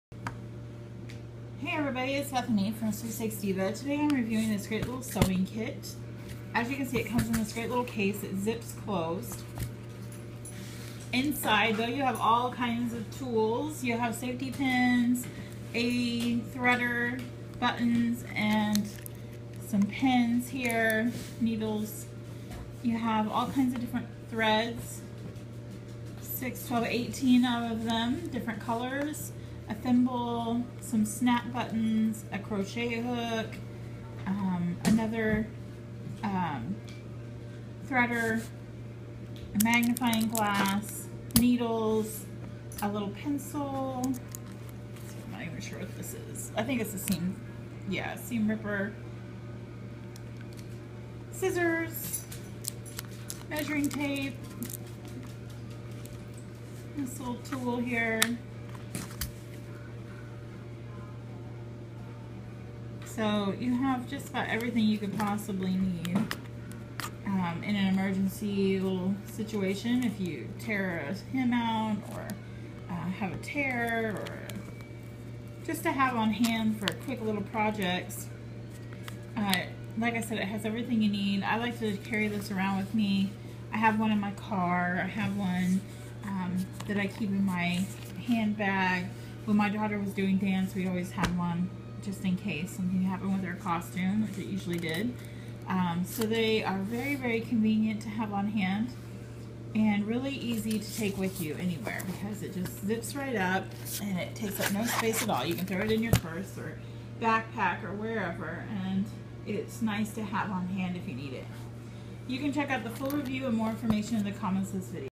PrezzyDeal Sewing Kit
I just got this awesome sewing set from PrezzyDeal. This one comes in a great little travel case. It folds in half and has a zipper so you can keep all items nice and safe inside. It comes with all kinds of sewing tools. It has needles, a needle threader, a package of buttons, safety pins, seam ripper, a variety of threads, scissors, crochet hook, snap buttons, pencil and a tape measure. This kit is a life safer when you rip your outfit or lose a button. My daughter is a dancer so we keep this kit in her dance box with everything else in case we have a tear, or missing button. I can pull this out and have her fixed up in no time flat. It’s also nice to just have on hand in you sewing kit. I love it!! I received this item complimentary or at a reduced price for my honest review. However, I only recommend products I truly love and use to my readers.